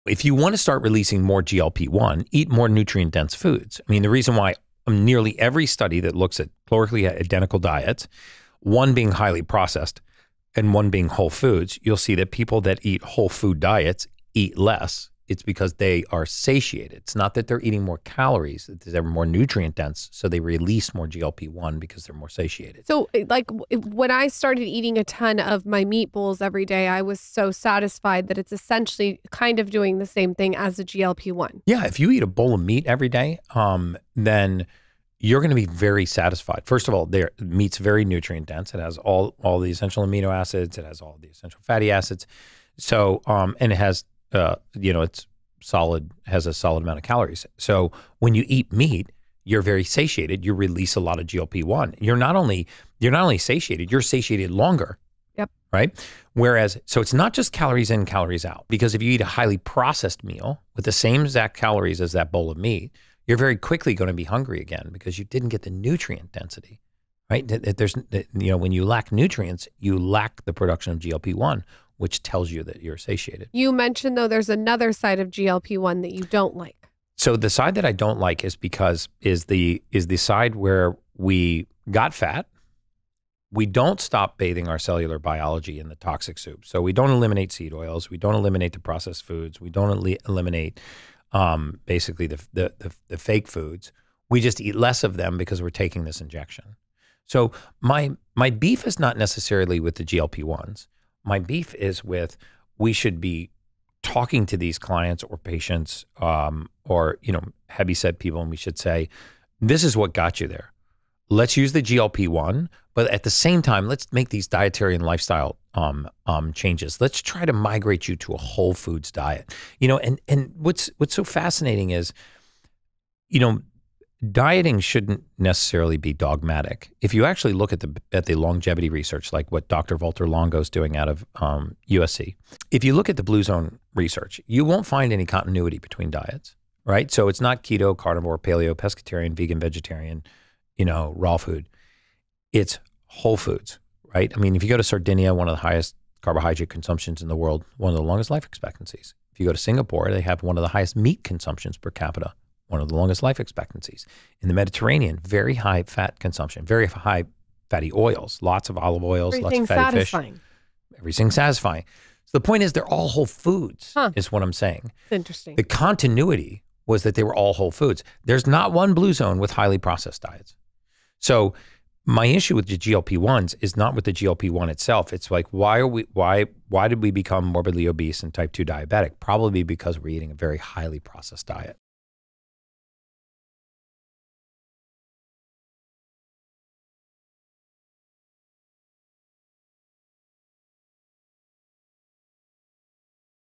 Expert Biohacker Gary Brecka: Natural GLP-1 Hacks, & Whole Food Diets
Join us as we sit down with Gary Brecka – a human biologist, health expert, entrepreneur, & host of The Ultimate Human Podcast. Known for his groundbreaking work in optimizing human performance & wellness, Gary is devoted to helping people unlock their full potential through science-backed approaches to health – focused on optimizing human genetics, hormones, & longevity. In this episode, Gary gets real about the essential vitamins everyone needs for optimal health, uncovers how nutrient deficiencies often mimic other conditions, reveals the truth about stubborn fat loss, & shares all you need to know in order to live a healthier, more vibrant life!